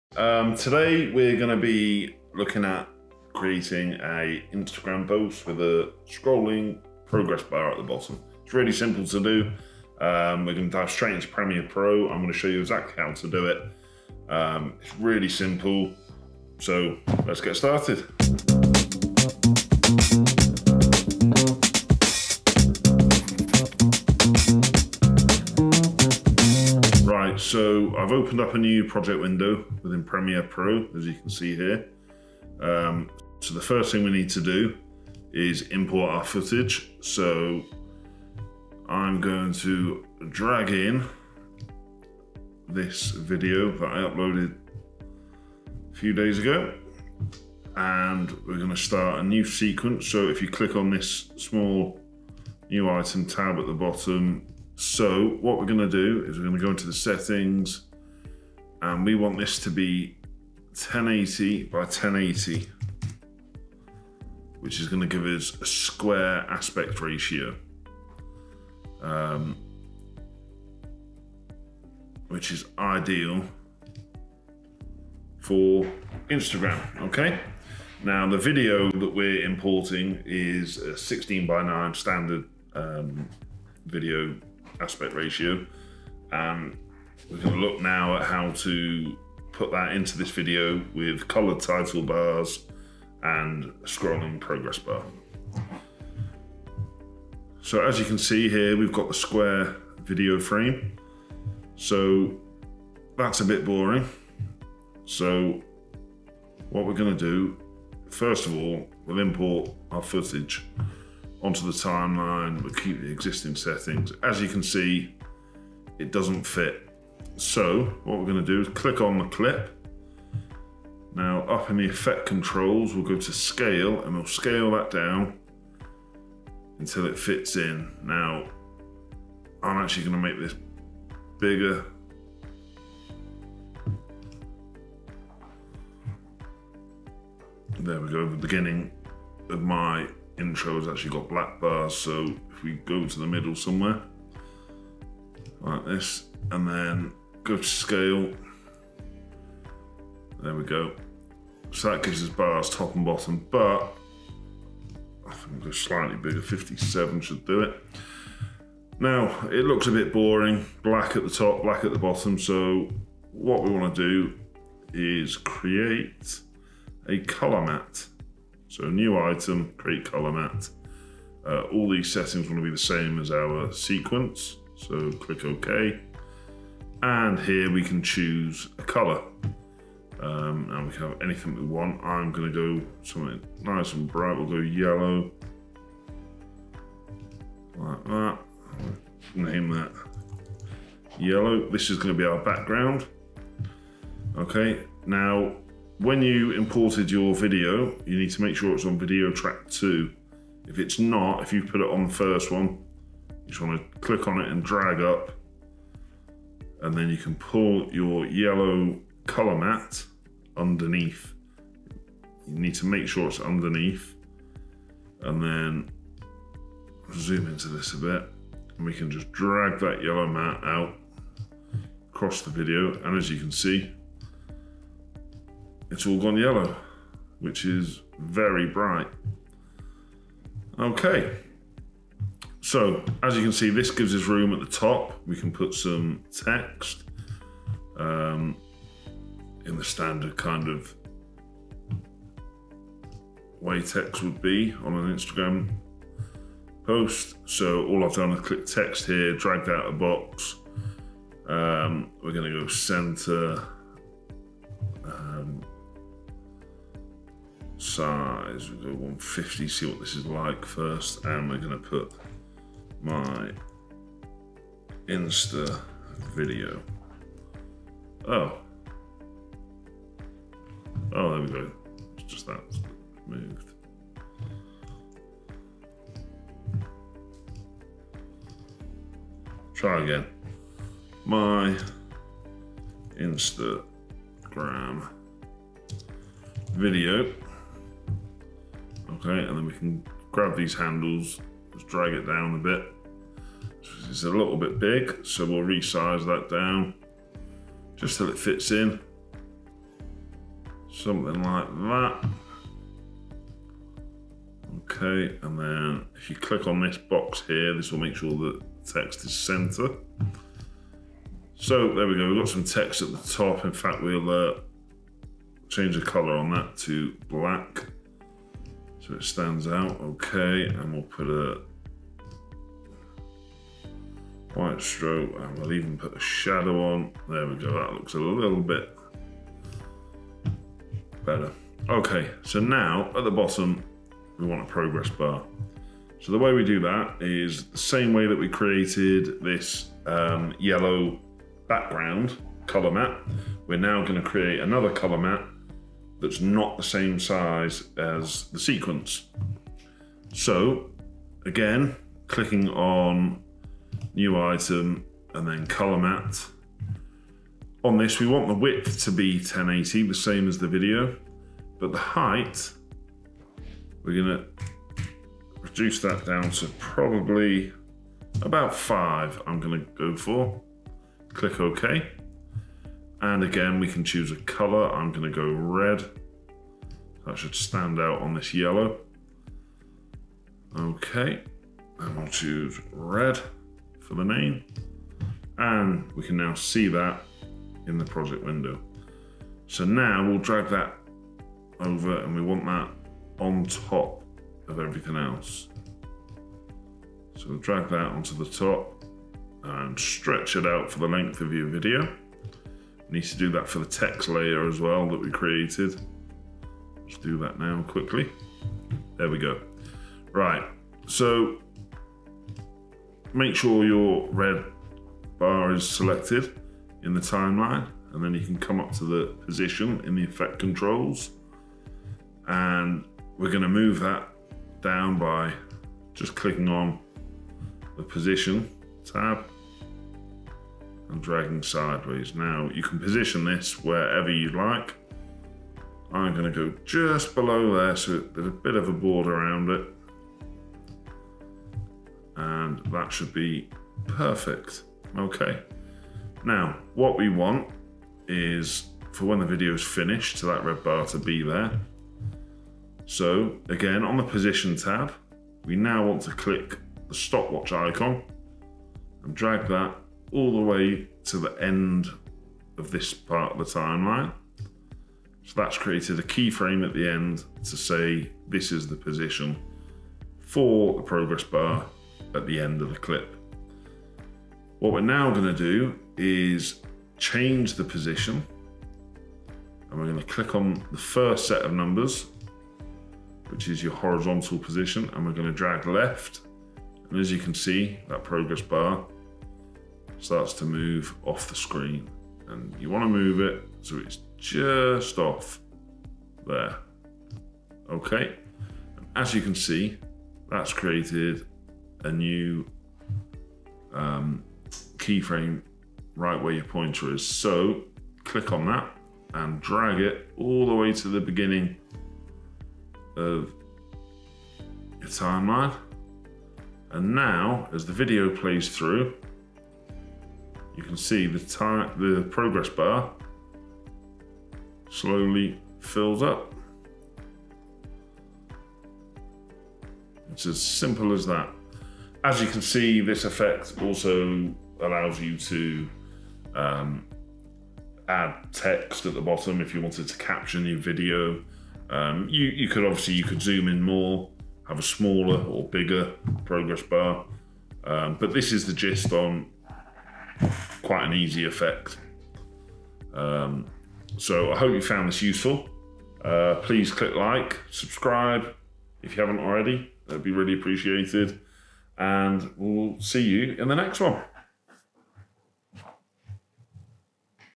How to add a progress bar to instagram videos
Easy method to edit a square aspect ratio video with top and bottom bars and scrolling progress bar